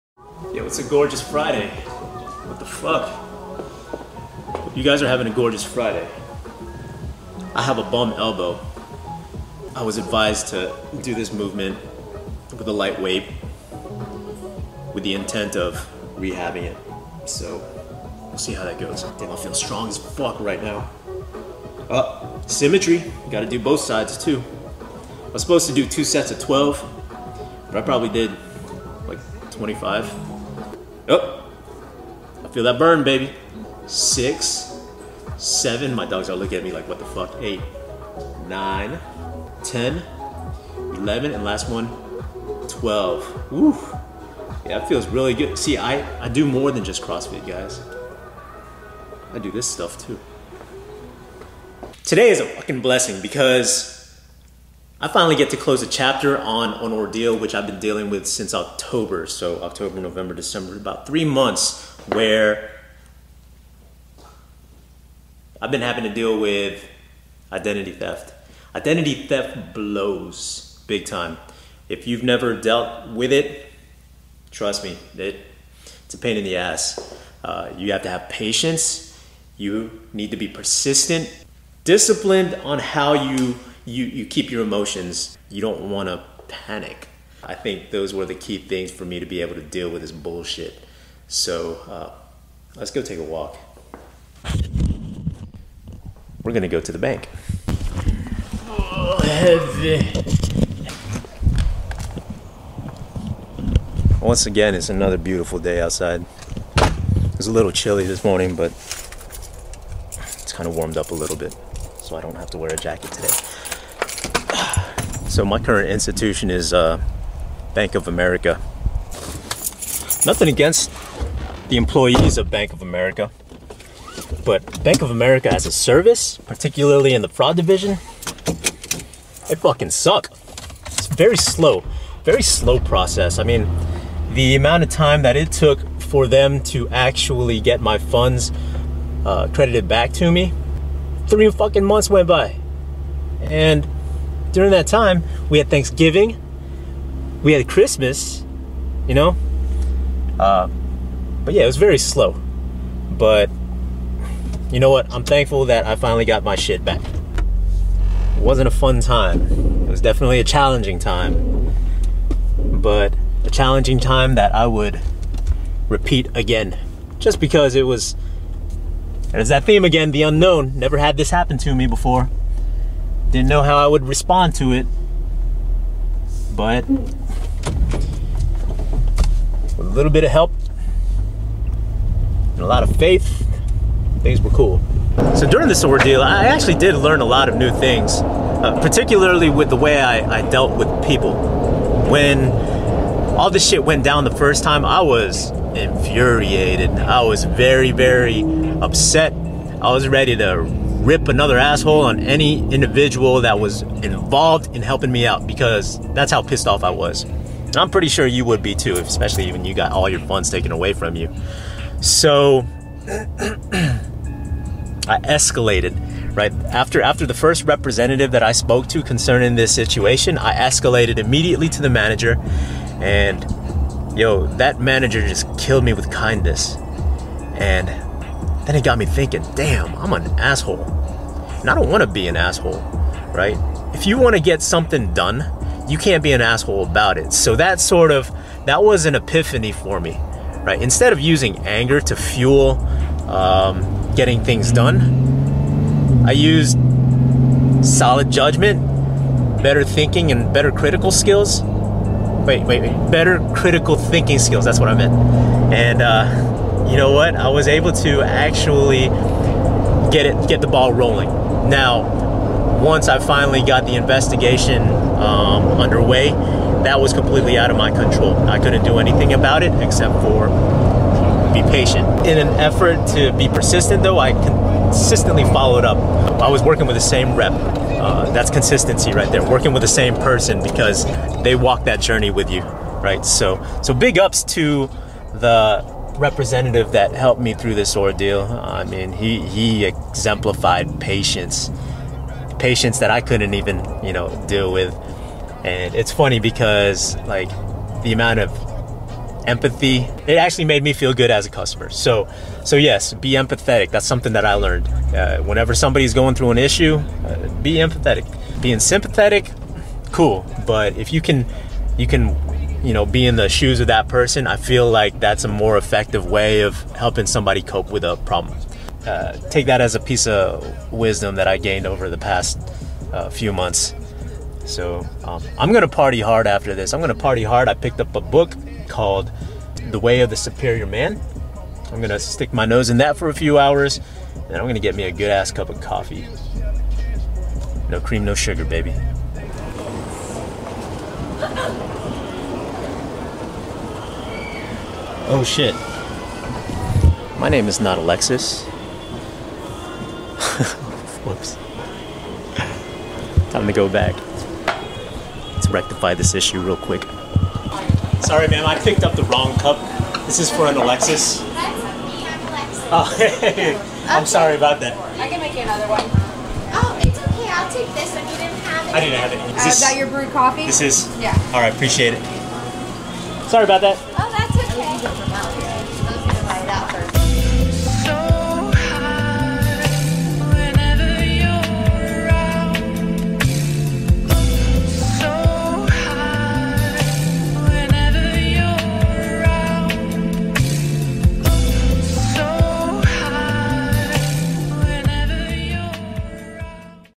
HADJI #002 - Dealing With Public Nudity
STRONG LANGUAGE.

The following  closes the chapter on how I dealt with public nudity. And the lessons learned.